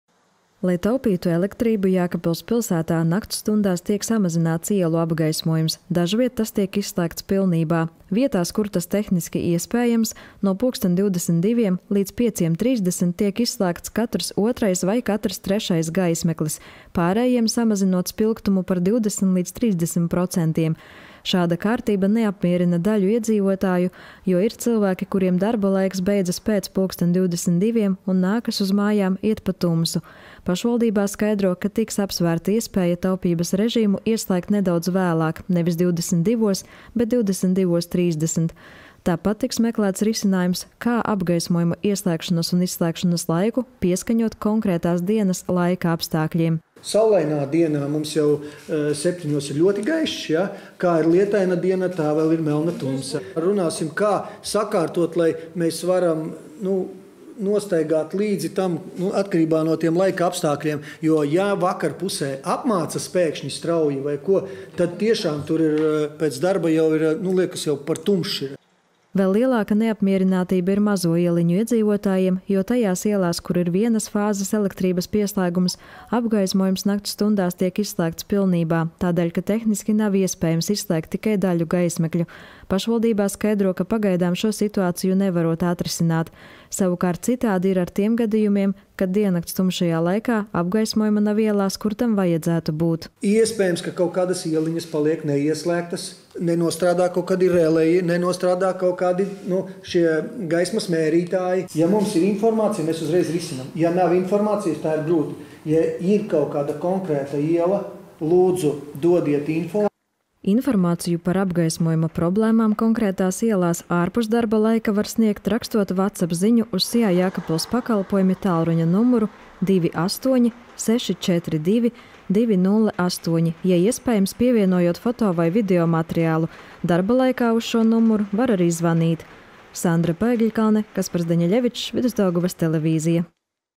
Jēkabpilī naktīs samazina ielu apgaismojumu, dažviet mazajās ieliņās to izslēdz pilnībā
Lai taupītu elektrību, Jēkabpils pilsētā nakts stundās tiek samazināts ielu apgaismojums, dažviet tas tiek izslēgts pilnībā. Vietās, kur tas tehniski iespējams, no pulksten 22:00 līdz 5:30 tiek izslēgts katrs otrais vai katrs trešais gaismeklis, pārējiem samazinot spilgtumu par 20 līdz 30 procentiem. Šāda kārtība neapmierina daļu iedzīvotāju, jo ir cilvēki, kuriem darba laiks beidzas pēc pulksten 22:00 un nākas uz mājām iet pa tumsu. Pašvaldībā skaidro, ka tiks apsvērta iespēja taupības režīmu ieslēgt nedaudz vēlāk – nevis 22:00, bet 22:30. Tāpat tiks meklēts risinājums, kā apgaismojuma ieslēgšanas un izslēgšanas laiku pieskaņot konkrētās dienas laika apstākļiem.

“Saulainā dienā jau 7:00 ir ļoti gaišs, bet lietainā dienā vēl ir melna tumsa,” saka Jēkabpils novada domes priekšsēdētājs Raivis Ragainis. “Runāsim, kā to sakārtot, lai varam sekot līdzi atkarībā no laika apstākļiem. Ja vakarpusē pēkšņi apmācas, tad tiešām jau šķiet, ka ir pārāk tumšs.”

Vēl lielāka neapmierinātība ir mazo ieliņu iedzīvotājiem, jo tajās ielās, kur ir vienas fāzes elektrības pieslēgums, apgaismojums nakts stundās tiek izslēgts pilnībā, tādēļ ka tehniski nav iespējams izslēgt tikai daļu gaismekļu. Pašvaldībā skaidro, ka pagaidām šo situāciju nevarot atrisināt. Savukārt citādi ir ar tiem gadījumiem, kad diennakts tumšajā laikā apgaismojuma nav ielās, kur tam vajadzētu būt.

“Iespējams, ka apgaismojums kādās ieliņās paliek neieslēgts, jo nenostrādā kaut kādi releji, nenostrādā kādi gaismas mērītāji,” skaidro Raivis Ragainis. “Ja mums ir informācija, mēs uzreiz problēmu risinām. Ja nav informācijas, tad ir grūti. Ja ir apgaismojuma problēmas kādā konkrētā ielā, lūdzu, dodiet informāciju!”

Informāciju par apgaismojuma problēmām konkrētās ielās ārpus darba laika var sniegt, rakstot WhatsApp ziņu uz SIA “Jēkabpils pakalpojumi” tālruņa numuru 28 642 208, ja iespējams, pievienojot foto vai video materiālu. Darba laikā uz šo numuru var arī zvanīt.

Sandra Paegļkalne